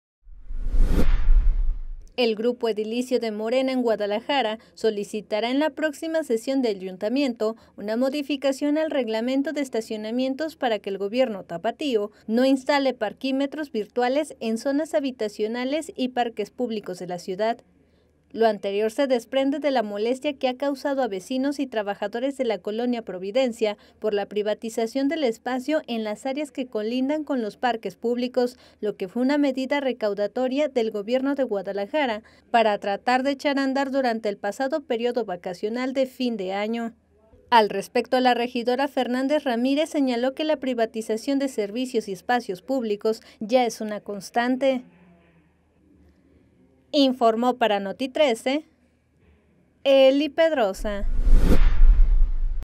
MÁS DE 3 MIL 200 ESTACIONAMIENTOS VIRTUALES SE HAN INSTALADO EN GUADALAJARA
Jalisco l Ante la falta de transparencia y de beneficio para el municipio, la regidora Mariana Fernández solicitará se revise la concesión a Parkimovil.